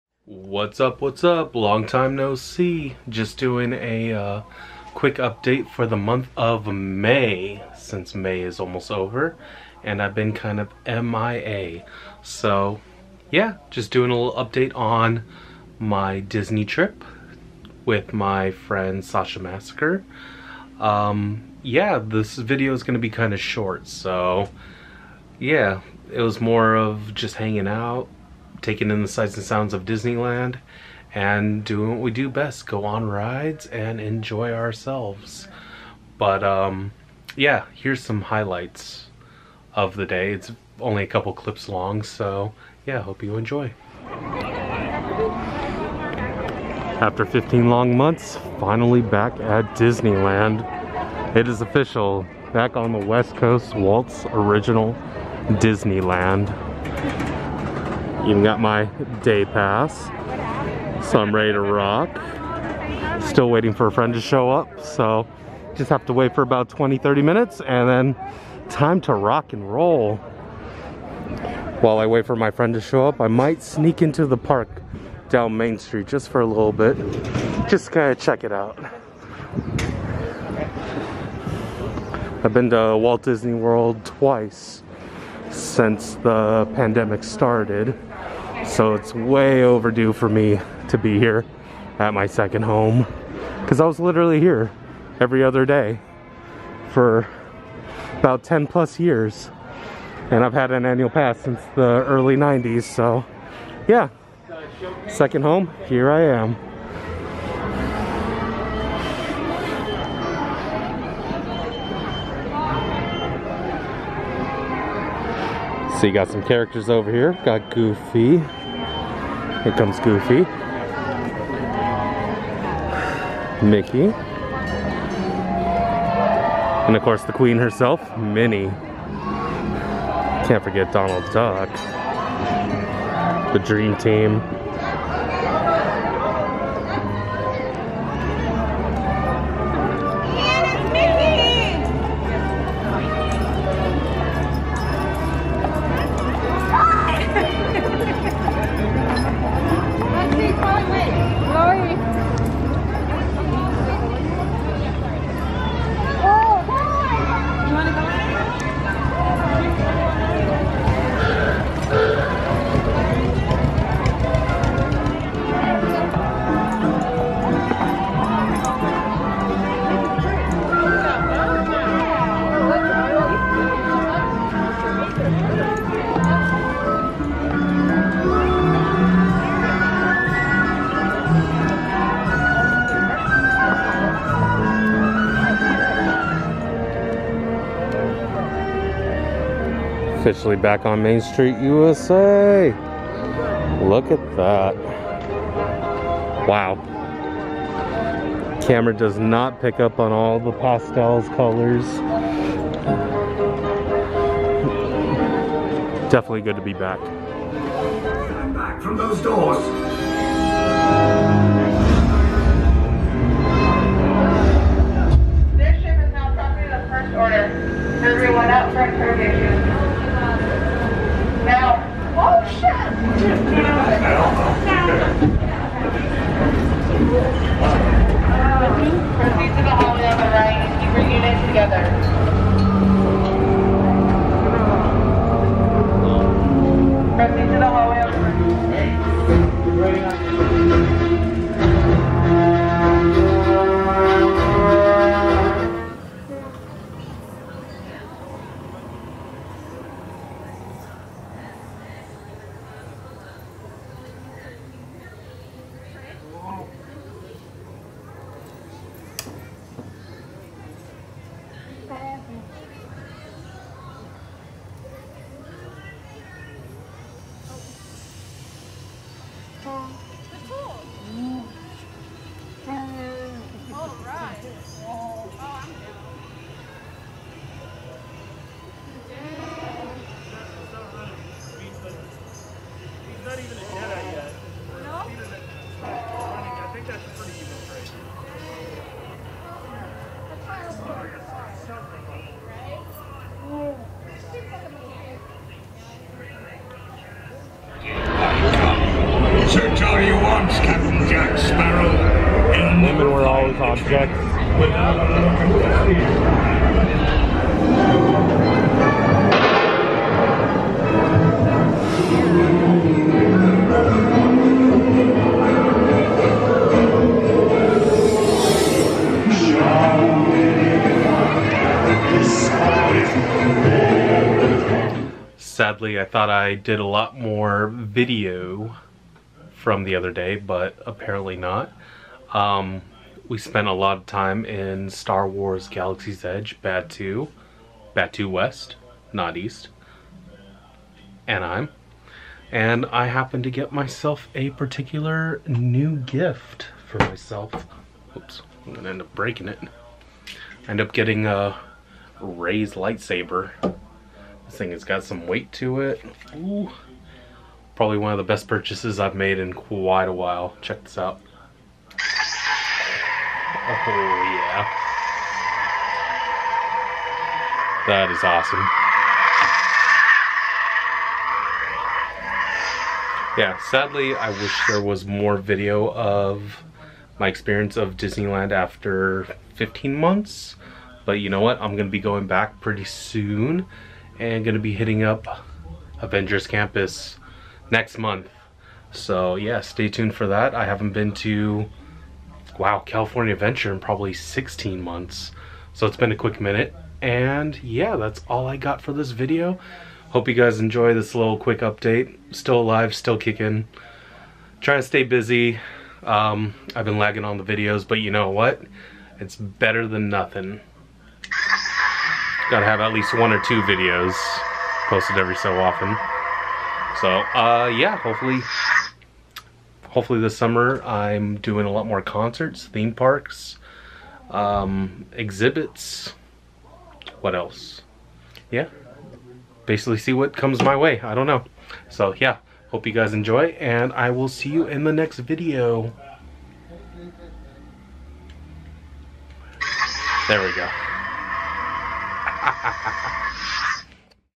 After 15 Months Im Back At DISNEYLAND!!!
After 15 long months Ive finally returned to Walts first Magic Kingdom DISNEYLAND. I felt like a million disney bucks being back in the theme park. Everything looked so fresh and new. Disneyland really is my second home and it was super missed.
Spending time with my bestie Sasha Massacre was much needed as well since for half a decade she's been my ride partner. At some point next month well be returning to Disney's California Adventure to check out Avengers Campus as well as the rest of the theme park.